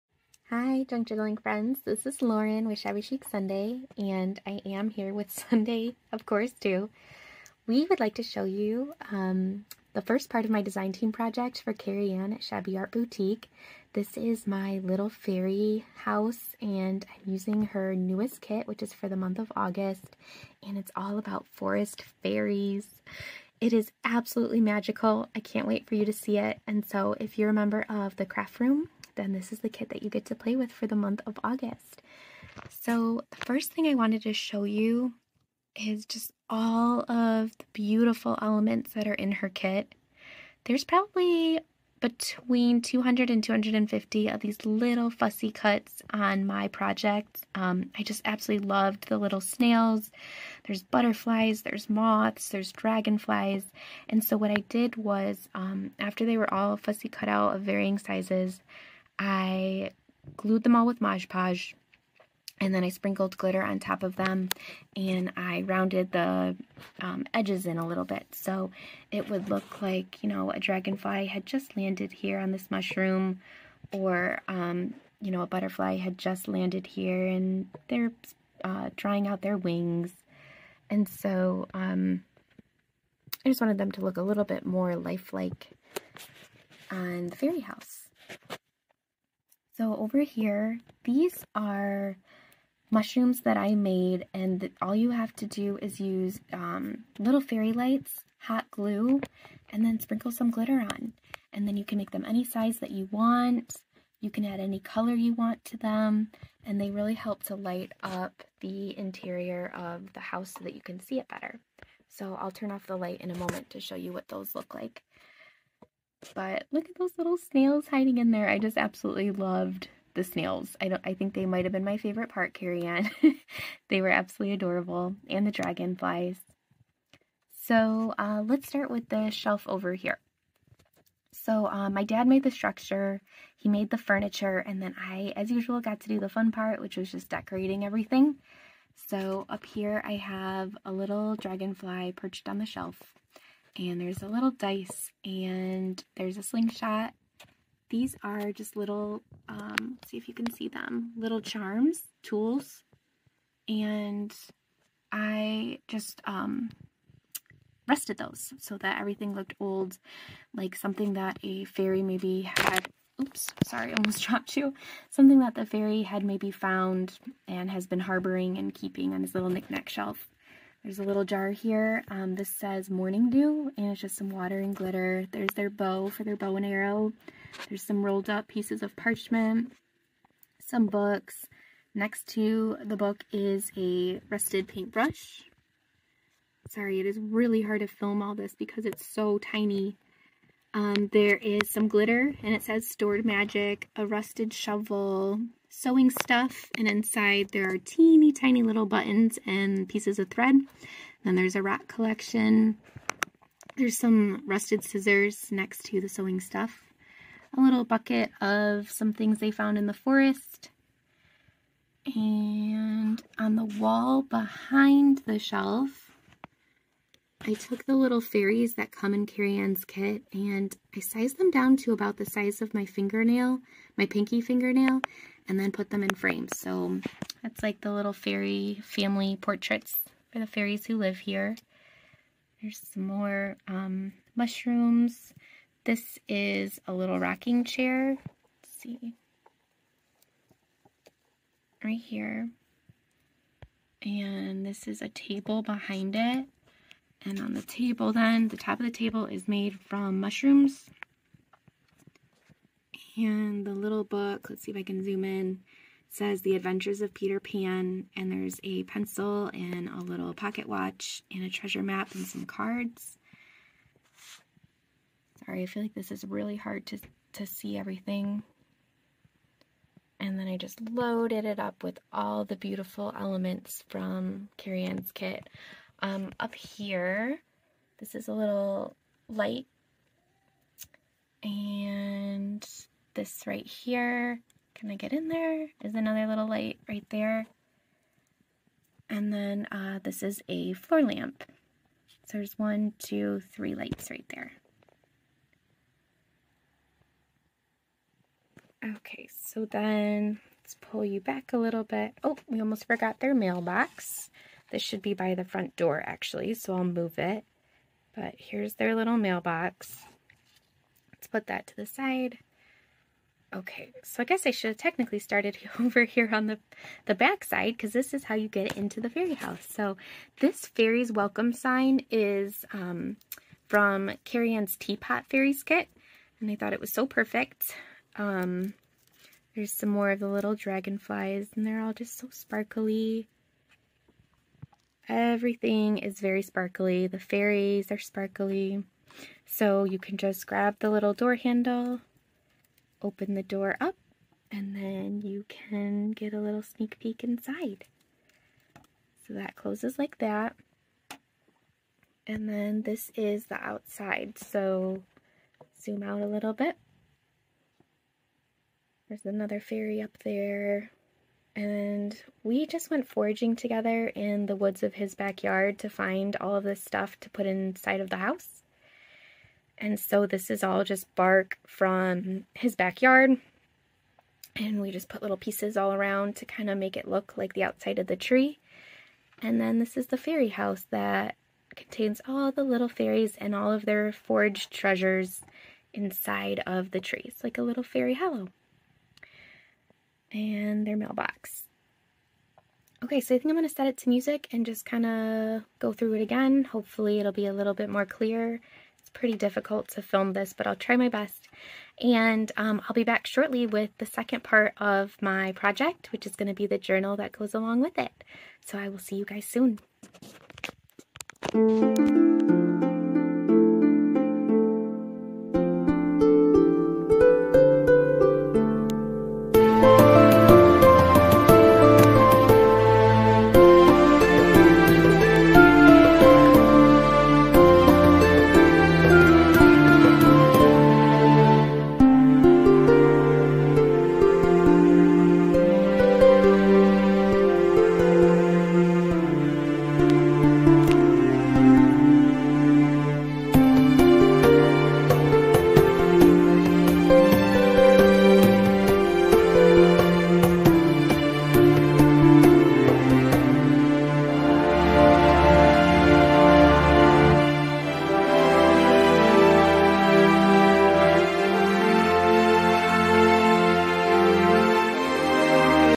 Magical Forest Fairy Tree House [DT project for shabbyartboutique ]
Hi, journaling friends! Thank you for stopping by today. Don't forget to say hello!

Materials used: The structure and all furniture is made entirely from natural items found in my backyard. Twigs, bark, mushrooms, acorns, etc. The structure is made from paper mache.

This is part one of my August DT project for @ShabbyArtBoutique using her kit, "Forest Faes" which is exclusive to members of the Craft Room.

Nicola: Myshabbycraftcorner    / @myshabbycraftcorner
Elisa’s Creations    / @elisascreations.
Angelique: Ang’ scrap Creations    / @vdwangie1974
Sandra: Ateliervintagerose    / @ateliervintagerose